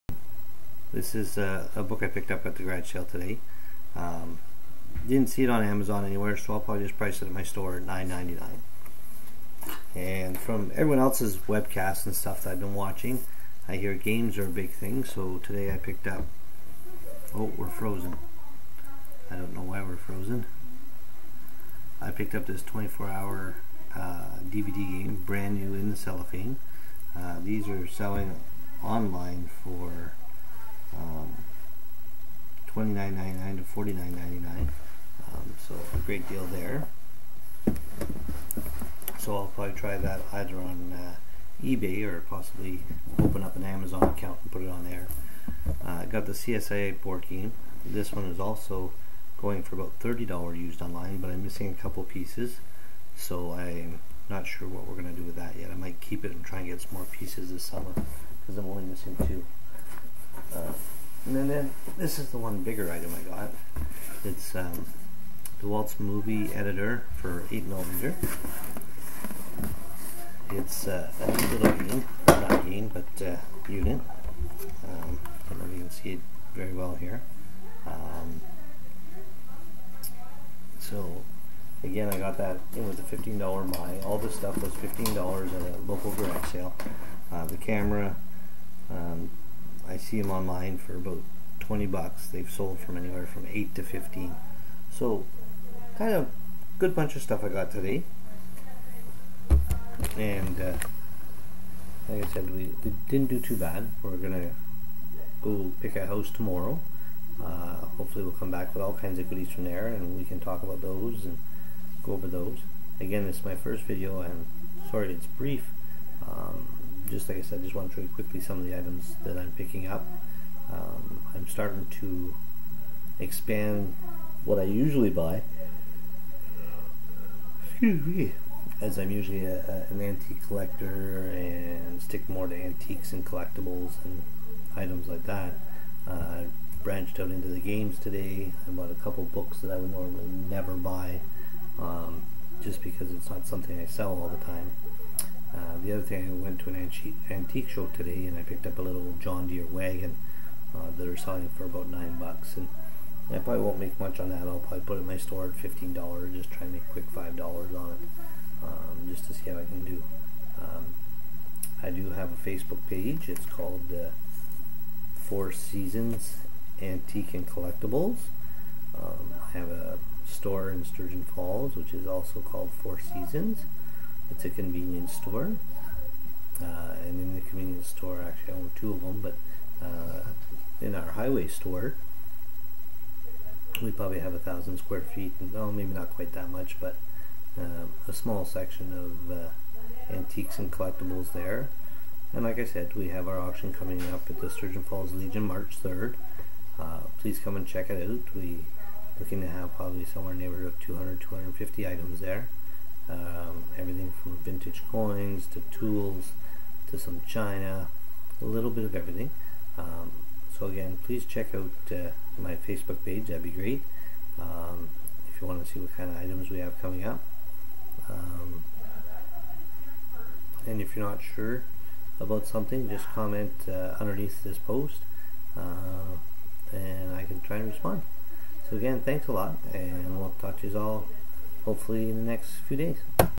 First Garage sale of the year Part 2
Sorry this was not in one video but the wife decide to have a chat in the middle.  Hope you enjoy this it is the completion of my first garage sale of the year.